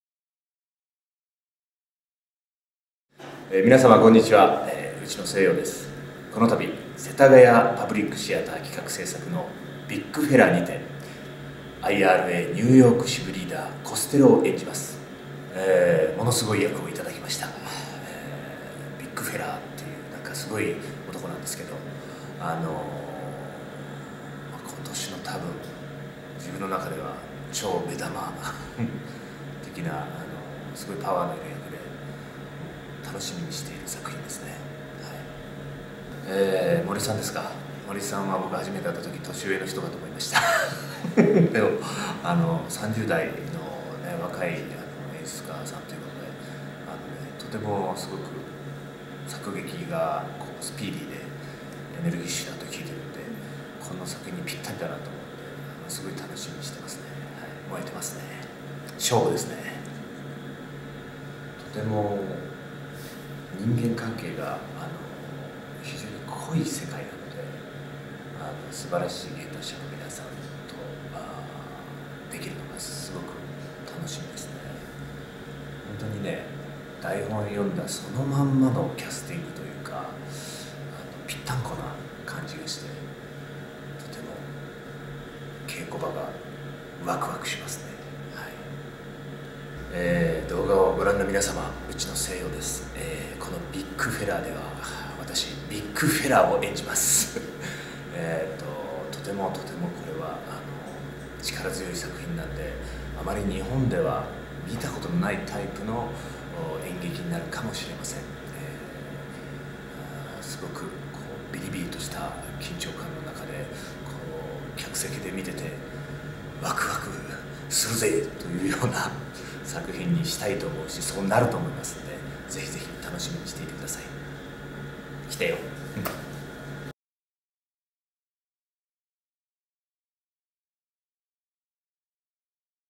『THE BIG FELLAH ビッグ・フェラー』内野聖陽さんからのメッセージ
2014年5〜6月、世田谷パブリックシアターにて『THE BIG FELLAH ビッグ・フェラー』上演！
内野聖陽さんからメッセージが届きました。

【公演概要】
201４年5月20日（火）～6月8日（日）
世田谷パブリックシアター

作：リチャード・ビーン
翻訳：小田島恒志
演出：森新太郎

出演：
内野聖陽 ／ 浦井健治 ／ 明星真由美　町田マリー
黒田大輔　小林勝也 ／ 成河

舞台はニューヨーク。1972年から2001年までの約30年間にわたる歳月の中に、ある特殊な使命を帯びた男たちのドラマが展開する『ビッグ・フェラー』。
「信念」を持って生きてきた人々が、その「信念」に、ある日ふと疑問を抱いてしまったら？　殺伐とした世界の中で、拠りどころとすべき本当のアイデンティティとは？　愛する人を犠牲にしてまで守らなければならない、彼らの正義とは？
シリアスなテーマを扱いながらも、台詞は躍動感にあふれ、時として喜劇的、ストーリー展開はミステリアス、そして最後にひかえる大どんでん返し！　イギリスの人気劇作家の作品が、内野聖陽はじめ魅力的な夢のキャスト陣と、今年の読売演劇大賞で、大賞・最優秀演出家賞を受賞した、演出家・森新太郎により本邦初演で上演されます。
2014年の初夏を飾る話題作にぜひご期待ください。

企画制作：世田谷パブリックシアター
主催：公益財団法人せたがや文化財団　　後援：世田谷区

【国内ツアー】2014年6〜7月
兵庫：兵庫県立芸術文化センター　阪急 中ホール
新潟： りゅーとぴあ　新潟市民芸術文化会館・劇場
豊橋：穂の国とよはし芸術劇場PLAT　主ホール
大津：滋賀県立芸術劇場びわ湖ホール　中ホール